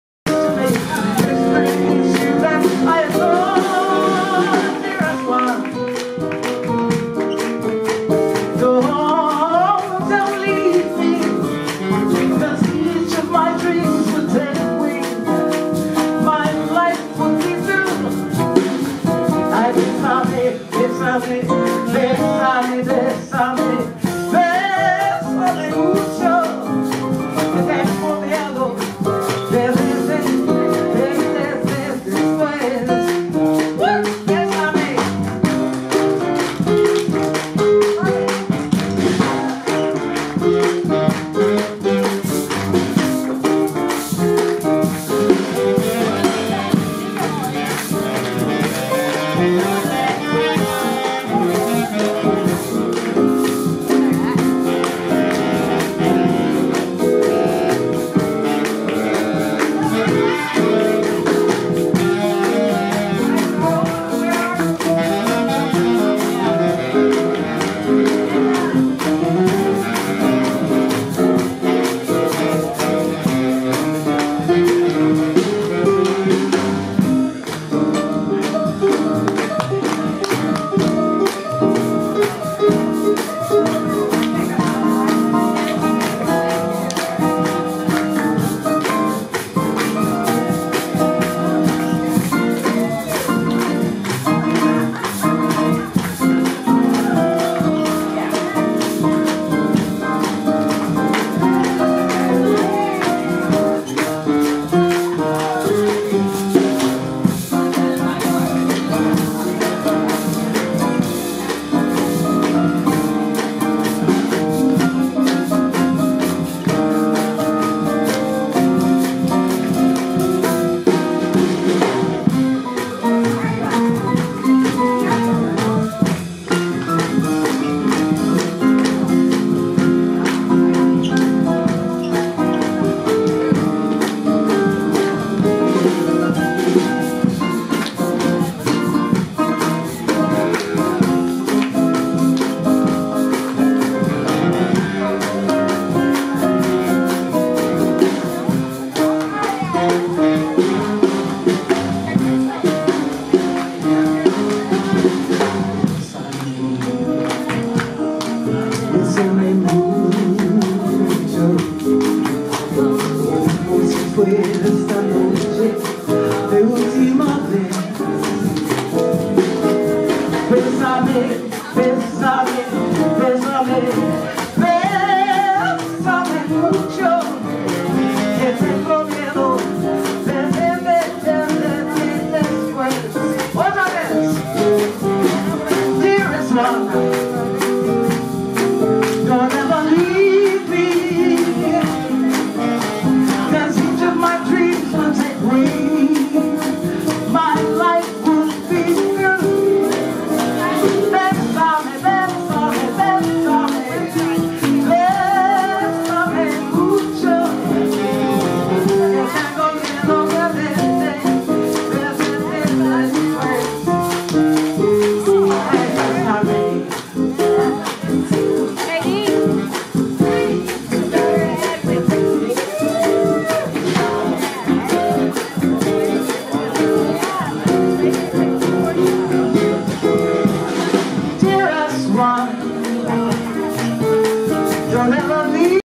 Nancy Morejón at Brava Theater 10.26.2017
Closing out a great lineup with Cuban poet Nancy Morejón and friends at Brava Theater in San Francisco.

Clip  P2490233